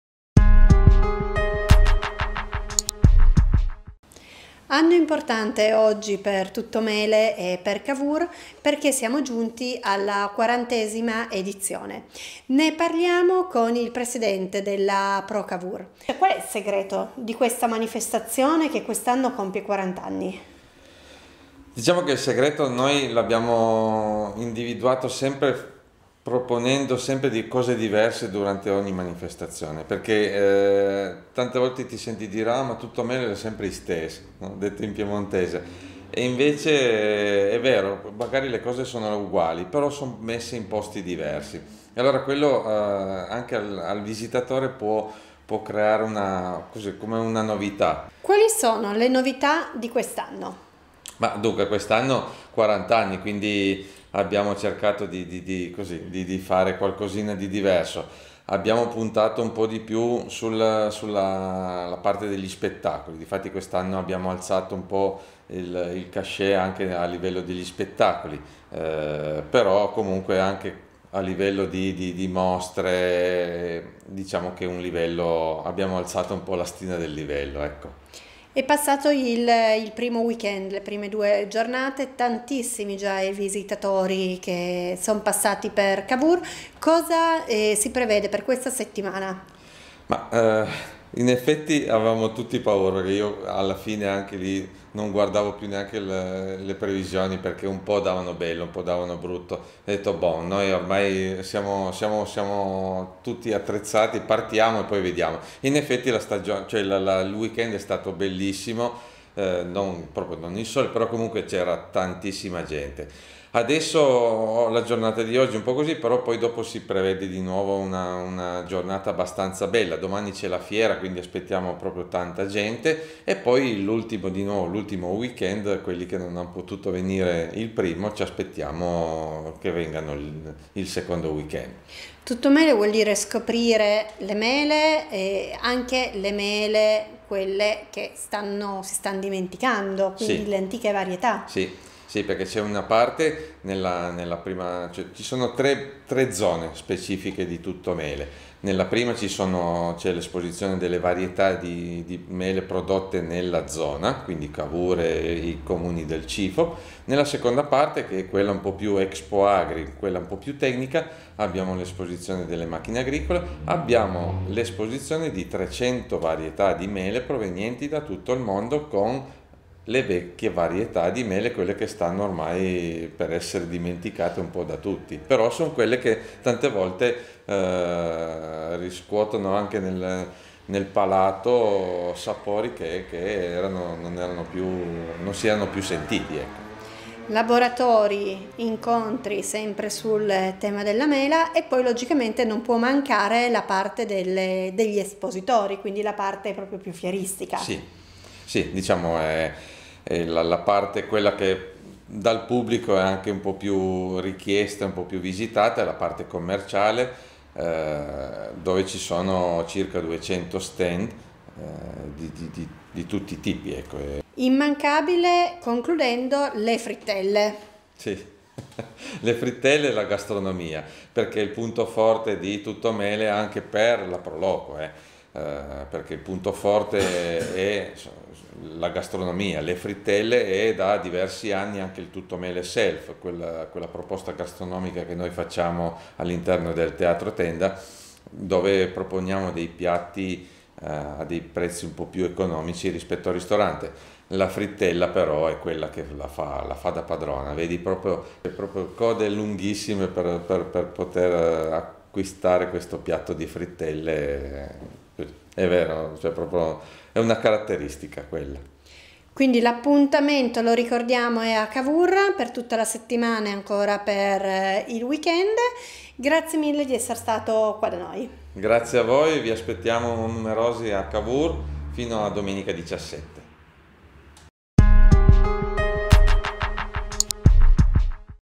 Tuttomele compie 40 anni
TUTTOMELE™ è la grande manifestazione autunnale che da 40 anni richiama l’attenzione di oltre 300.000 visitatori offrendo una straordinaria mostra mercato regionale di frutticoltura, gastronomia ed artigianato. Stand e spettacoli e, per l’occasione, tutti i ristoranti propongono menu solo a base di mele.

Incredibili sono le qualità e le quantità di mele provenienti da tutto il mondo che vengono esposte durante la festa.

Nel video l'intervista al presidente della ProCavour, Marcello Bruno.